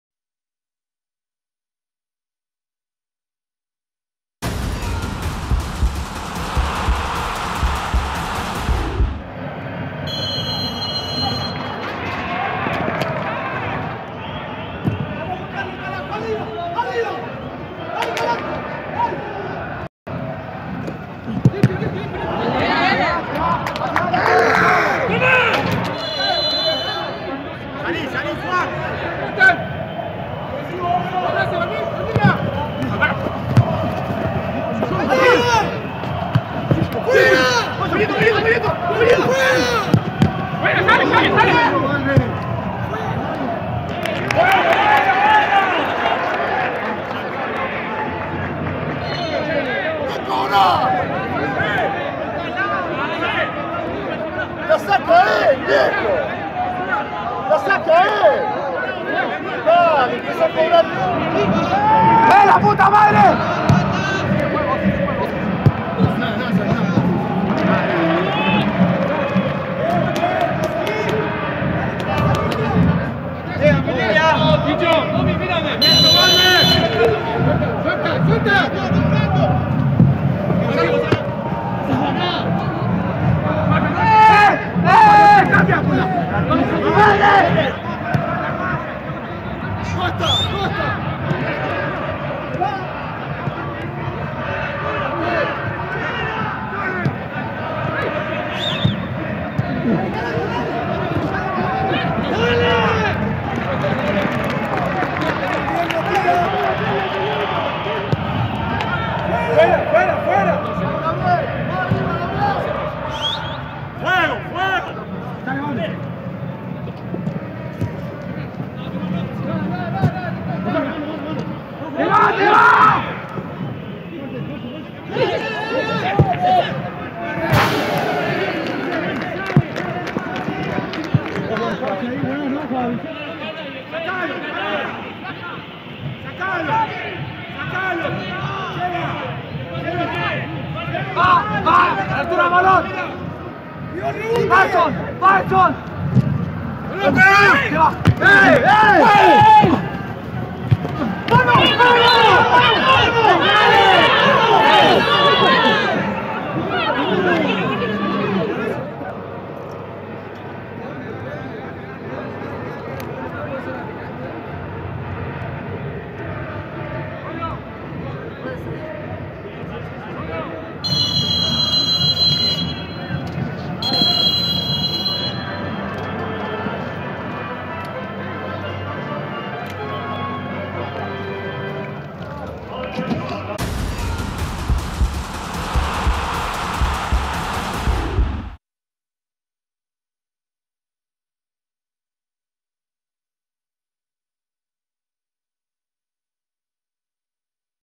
Universidad Católica 0-0 Colo Colo
⚽ Intenso empate en San Carlos de Apoquindo entre Universidad Católica y Colo Colo, que comprime más la tabla de posiciones.

📲💻📺 Revisa los goles gracias a TNT Sports.